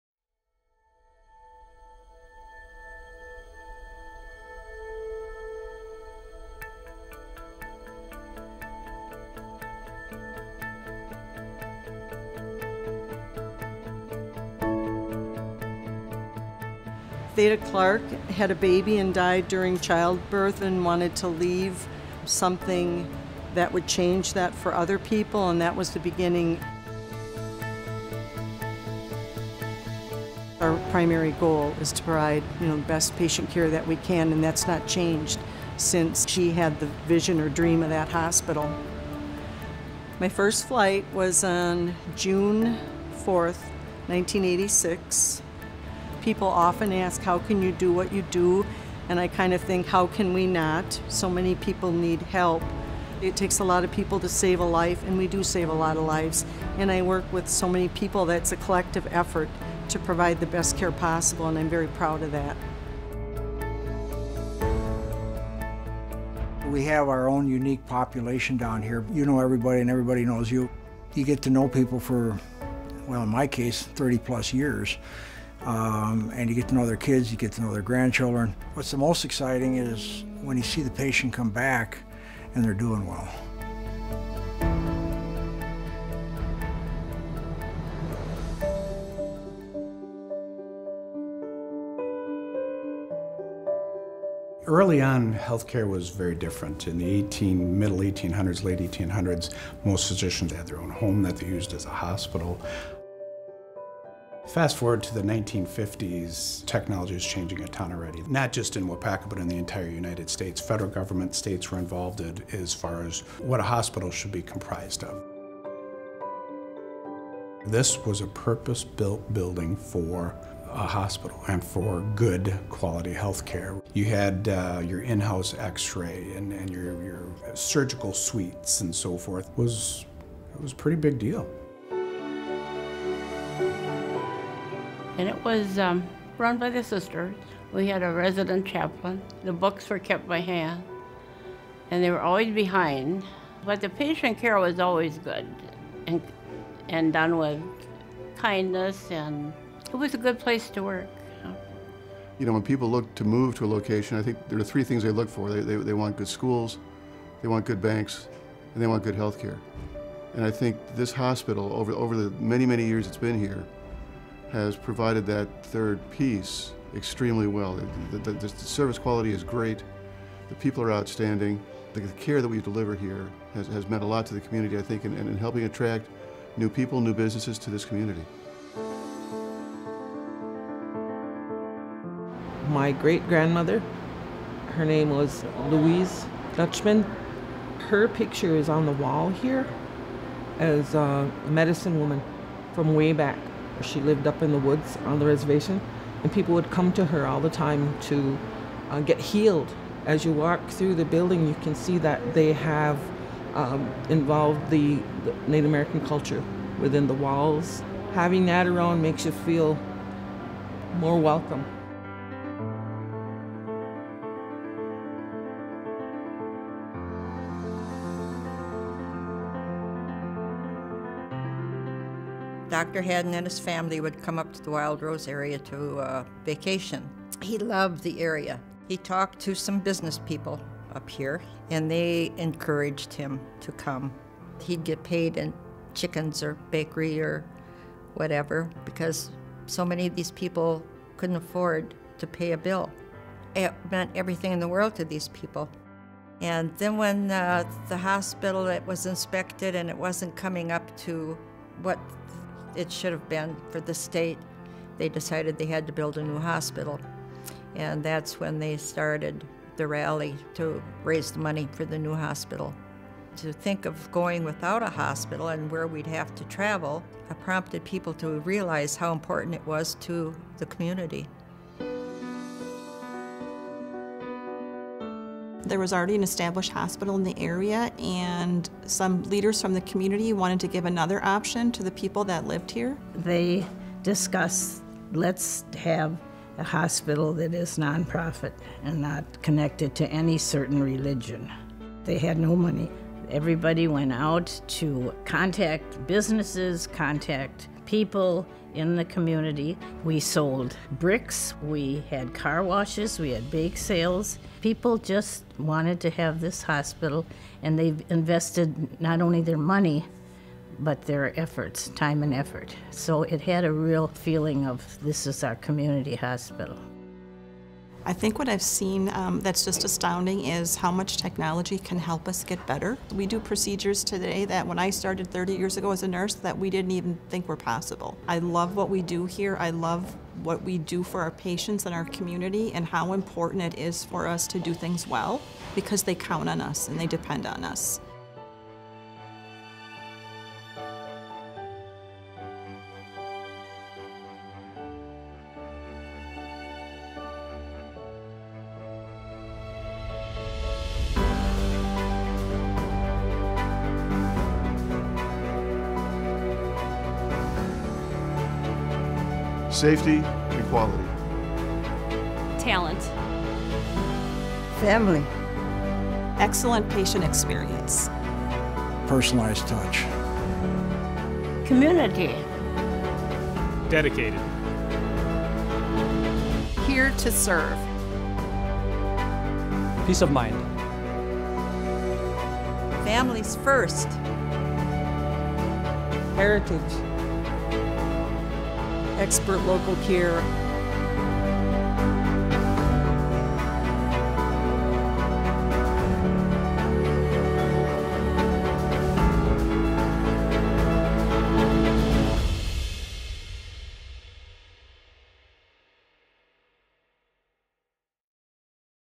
2019 ThedaCare History Video Part 2
We have a proud past, a promising future and a unique legacy that sets us apart from other healthcare organizations. Learn more about ThedaCare as we celebrate our 110th year of expert, local care. This second legacy video celebrates the history of our seven hospital communities.